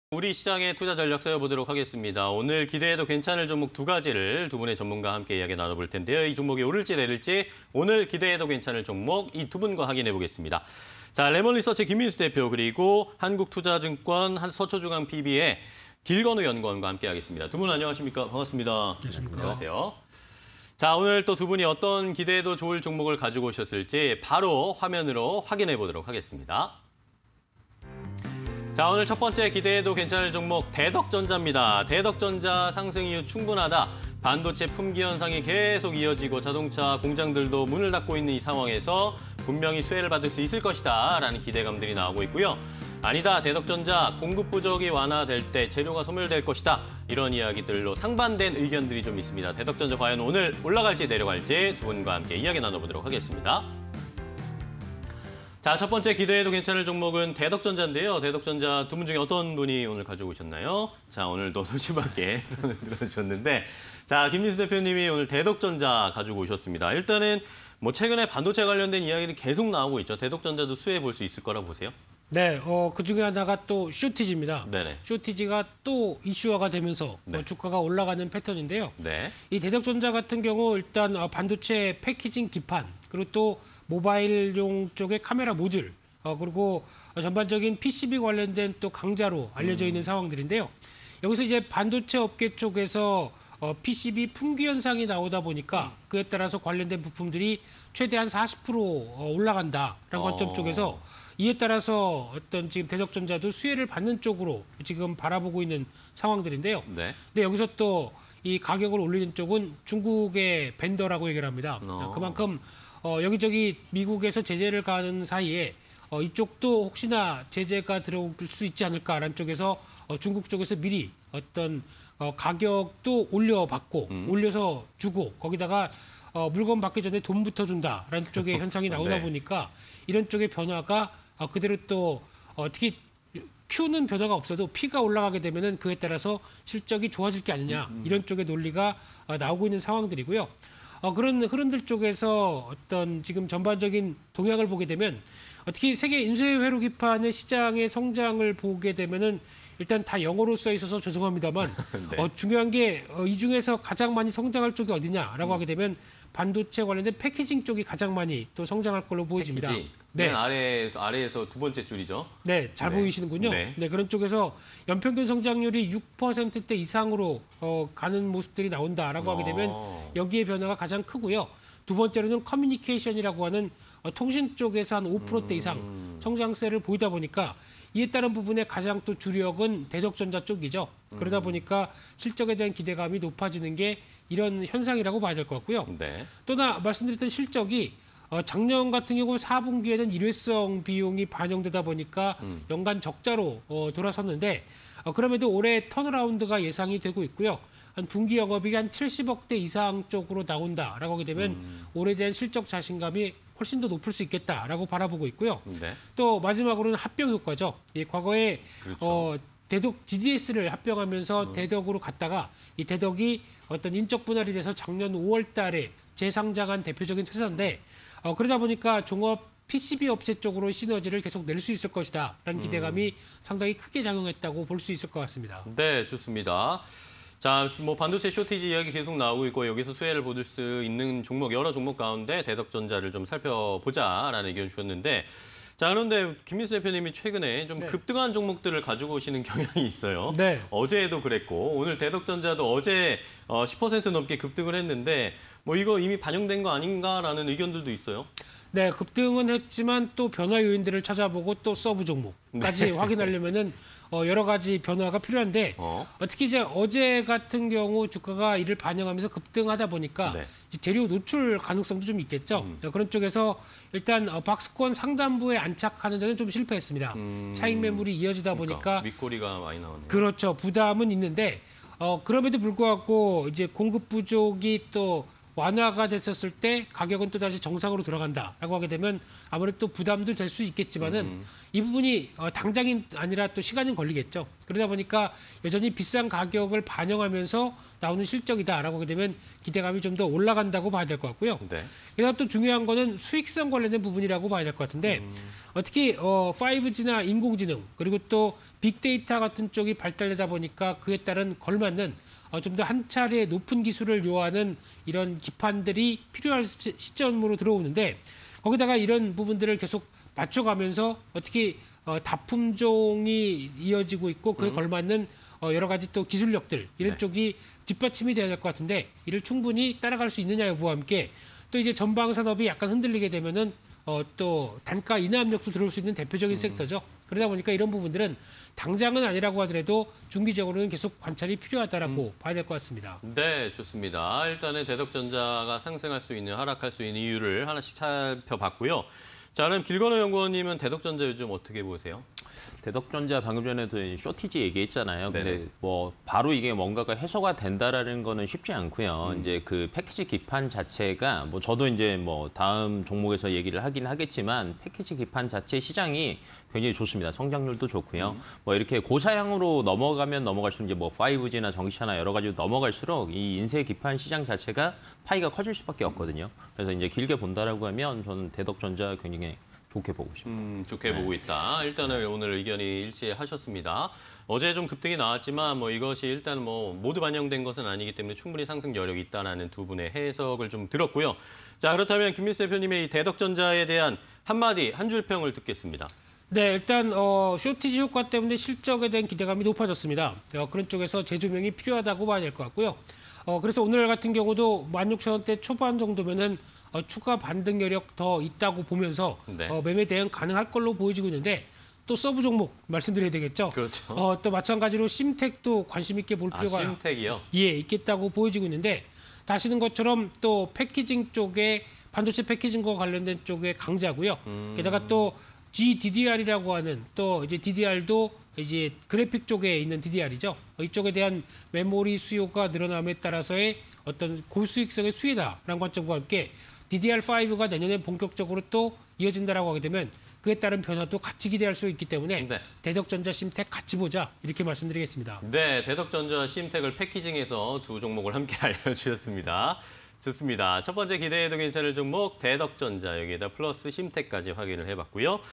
대덕전자 (353200)... 반도체 품귀현상 수혜 지속 VS 공급 부족 완화 시, 재료 소멸_기대해도 괜찮아 (20210420)
대덕전자 (353200) 오른다: 반도체 품귀현상 수혜 지속
- 반도체 업계 인쇄회로기판(PCB) 품귀현상
- 고사양 칩 패키징용 기판 부품가격 폭등
- 올해 실적 턴어라운드 기대
- 분기 영업이익 70억 돌파 여부 체크
- 대덕GDS합병으로 시너지 효과 발현

# 대덕전자 (353200) 내린다: 공급 부족 완화 시, 재료 소멸
- 호재 속 박스권 상단부 돌파 시도
- 공급 부족 완화 시, 매출 하락 우려
- 전방산업 변화 시, 단가 인하 가능성
- 향후 수익성 지속여부 관심